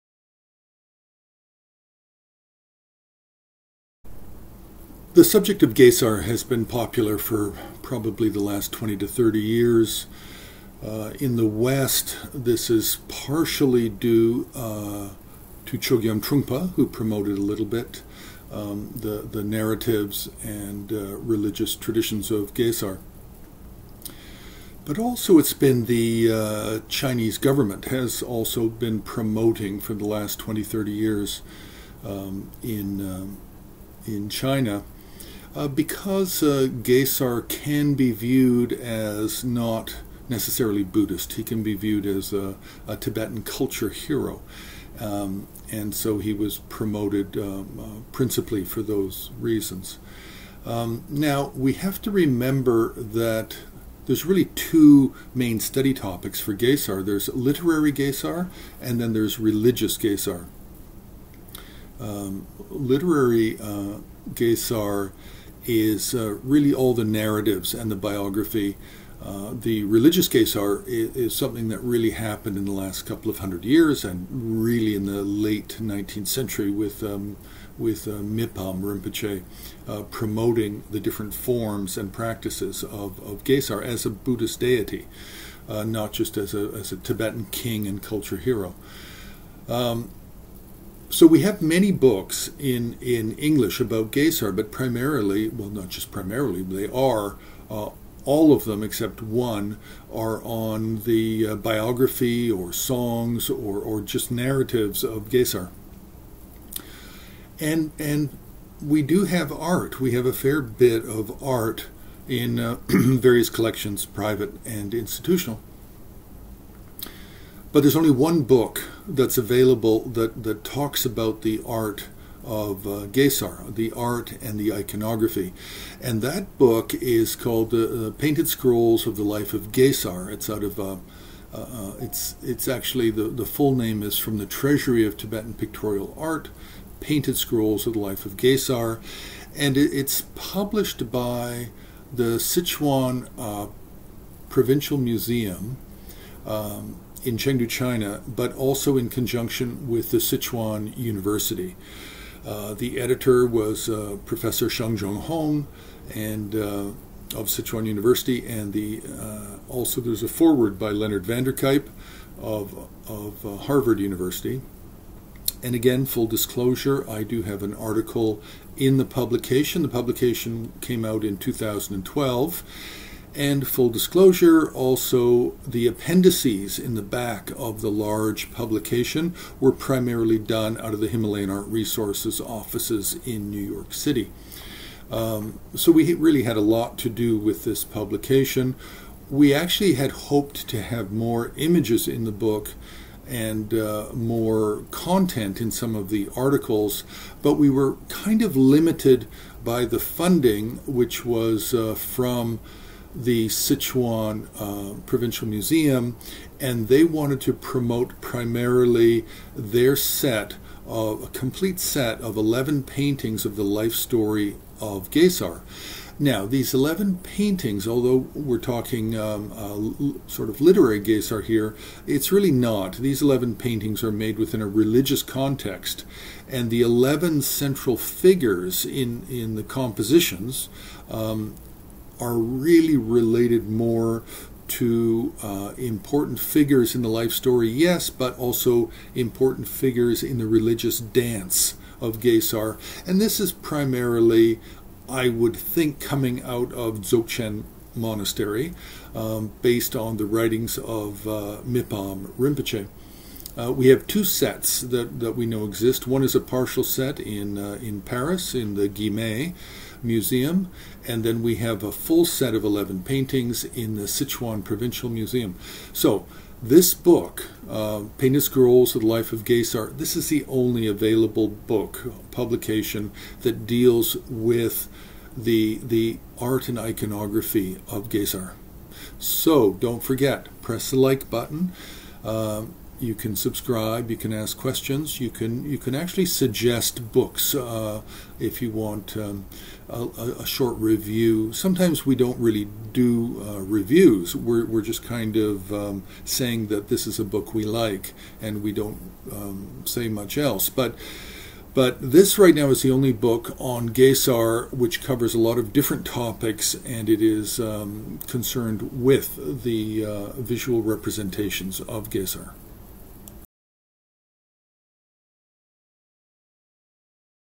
Painted Scrolls of the Life of Gesar
From the Treasury of Tibetan Pictorial Art: Painted Scrolls of the Life of Gesar. Editor-in-Chief, Zhang Changhong. Forward by Leonard van der Kuijp. Article by Jeff Watt. Sichuan Museum, 2012. ISBN: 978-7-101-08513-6.